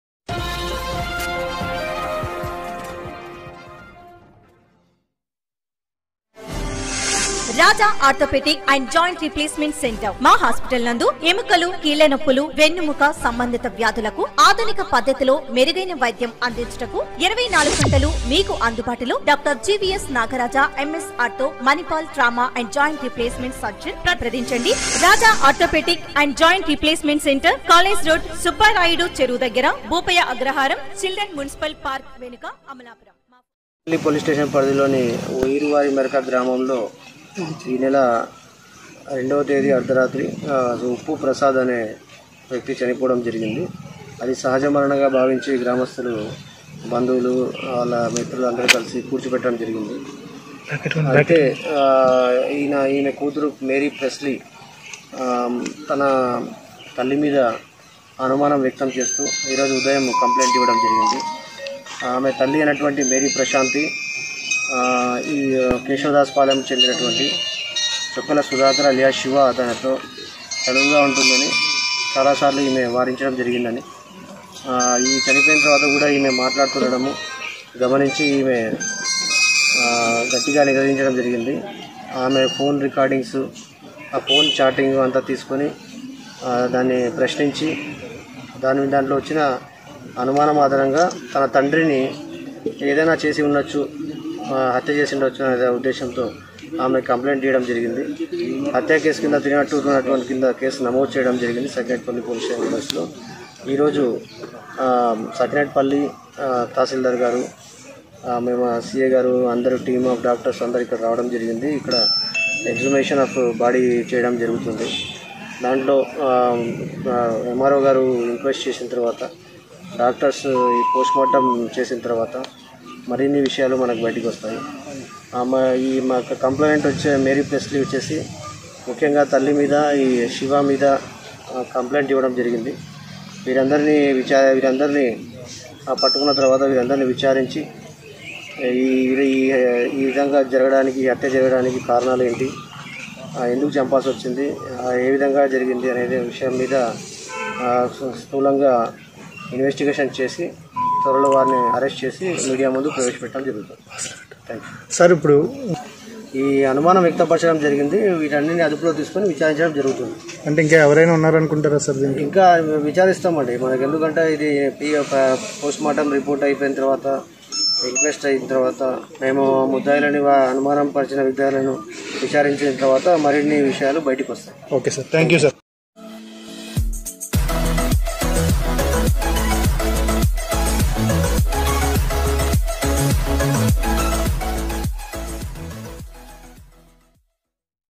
దారుణం : ప్రియుడి కోసం భర్తను హతమార్చిన భార్య II TEJA TIMES II ఉయ్యూరి వారి మెరకలో ఘటన
తూర్పు గోదావరి జిల్లా.. సఖినేటిపల్లి మండలం వుయ్యూరు వారి మెరకలో దారుణం. అక్రమ సంబంధానికి అడ్డుగా ఉన్నాడని ప్రియుడు చొప్పల్ల శివ సహకారంతో భర్త ఉప్పు ప్రసాద్ ను హతమార్చిన భార్య ప్రశాంతి.
ఆలస్యంగా వెలుగులోకి ఘటన.

ఈనెల రెండో తారీఖున అర్ధరాత్రి ప్రసాద్ చనిపోవడంతో.. సహజ మరణంగానే భావించి, అంత్యక్రియలు నిర్వహించిన గ్రామస్తులు.

పదిహేను రోజుల తర్వాత బయటపడ్డ ఫోన్ సంభాషణల ఆధారంగా, హత్య చేసిన విధానం బయట పడటంతో, ఆడియో రికార్డింగ్ ల ఆధారంగా గ్రామస్తులు పోలీసులకు ఫిర్యాదు.

ప్రియుడు చొప్పల్ళ శివ, ప్రశాంతిని అదుపులోనికి తీసుకుని విచారిస్తోన్న పోలీసులు.

మృతుడి శవాన్ని బయటకు తీసి  పోస్టుమార్టం నిర్వహించనున్న పోలీసులు.